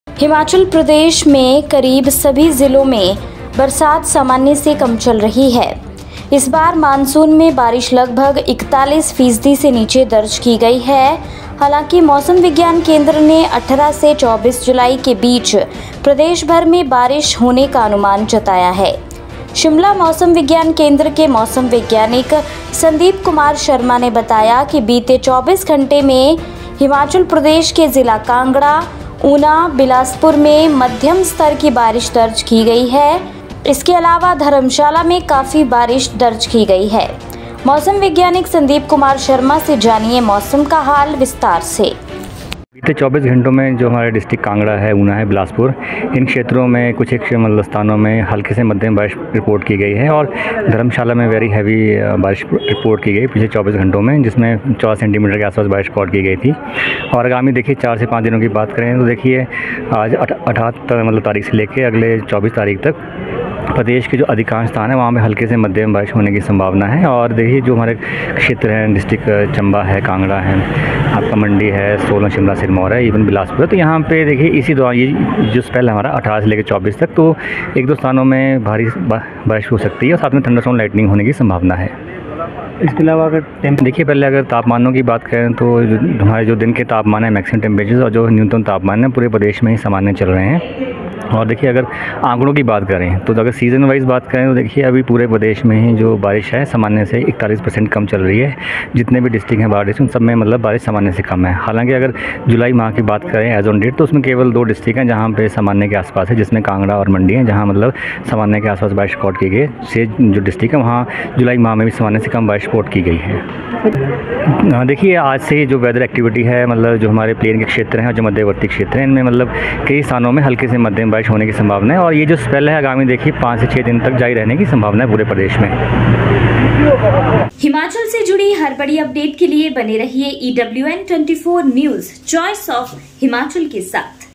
हिमाचल : मानसून की रफ्तार सुस्त, क्या कहते हैं मौसम वैज्ञानिक-देखें | Monsoon | Himachal | Rain
Rainfall is less than normal in almost all the districts of Himachal Pradesh. This time the monsoon rainfall has been recorded below 41 percent. However, the Meteorological Center has predicted rain across the state between 18 and 24 July. Meteorologist Sandeep Kumar Sharma of Shimla Meteorological Center said that in the last 24 hours, moderate rain has been recorded in Kangra, Una and Bilaspur districts of Himachal Pradesh. Apart from this, a lot of rain has been recorded in Dharamsala.